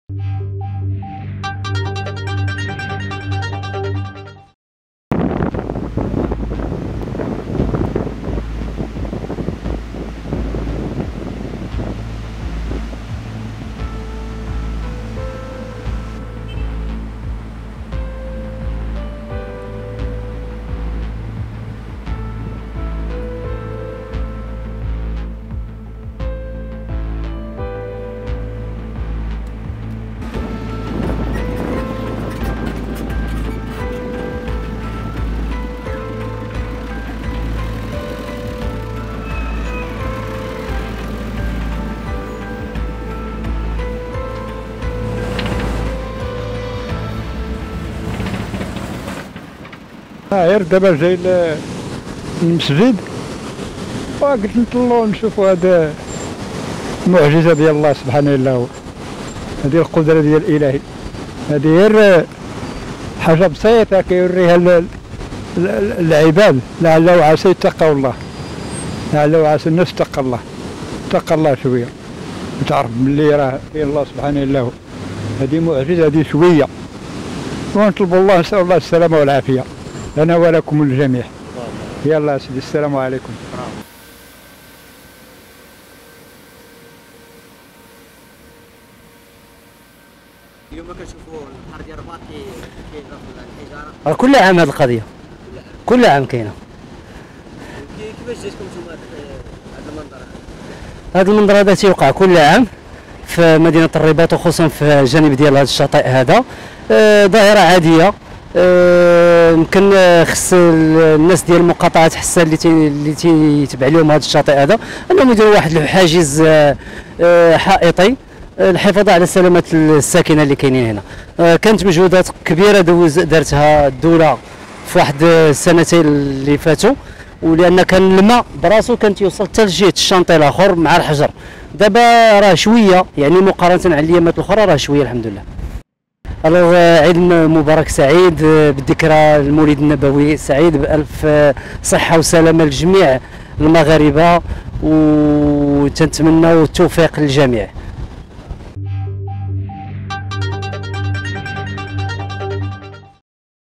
مشهد خطير..ميني تسونامي ضرب شاطئ الرباط والبحر لاح الحجر..شوفو شنو وقع
رصدت شوف تيفي الأجواء بشاطئ الرباط تزامنا مع ارتفاع الأمواج.